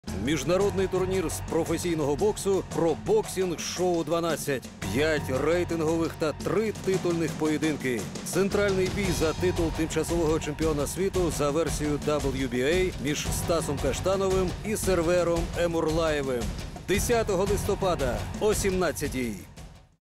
PRO BOXING SHOW XII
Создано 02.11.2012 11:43

Боксерская промоутерская компания "Union Boxing Promotion" 10 ноября 2012 г. проведет широкомасштабный турнир по профессиональному боксу "Pro Boxing Show 12", в рамках которого состоится три титульных и пять рейтинговых поединков.

+90,7 кг

Константин ГАВРИЛОВ

Украина

Юрий ВАЙНИЛЕНКО

Украина
4

раунда

50,8 кг
Галин ПАУНОВ

Болгария
Артем ДАЛАКЯН

Украина,

«Union Boxing Promotion»

6

раундов

50,8 кг
Геннадий ДЕЛИСАНДРУ

Украина
Александр ГРИЩУК

Украина,

«Union Boxing Promotion»

8

раундов

55,3 кг
Юрий ВОРОНИН

Украина
Александр ЕГОРОВ

Украина,

«Union Boxing Promotion»

8

раундов

69,9 кг
Семен МОРОШЕК

Латвия
Артем КАРПЕЦ

Украина,

«Union Boxing Promotion»

10 раундов

Бой за титул Интерконтинентального чемпиона по версии WBA

57,2 кг
Франклин ТЕРАН

Колумбия
33-6-1, 23 КО
Олег ЕФИМОВИЧ

Украина,

«Union Boxing Promotion»

22-2-0, 11 КО
12

раундов

90,7 кг
Исмаил АБДУЛ

Бельгия
46-24-2, 18 КО
Яго КИЛАДЗЕ

Украина,

«Union Boxing Promotion»
19-0-0, 13 КО
12

раундов

Бой за титул временного Чемпиона Мира по версии WBA

76,2 кг
Сервер ЭМУРЛАЕВ

Украина,

«Union Boxing Promotion»

22-0-0, 8 КО
Станислав КАШТАНОВ

Украина,

«Union Boxing Promotion»

29-1-0, 16 КО
12 раундов

Супервайзер рейтинговых поединков -- Михаил Завьялов (Украина, Киев)

Супервайзер титульных поединков -- Марьяна Борисова (Болгария)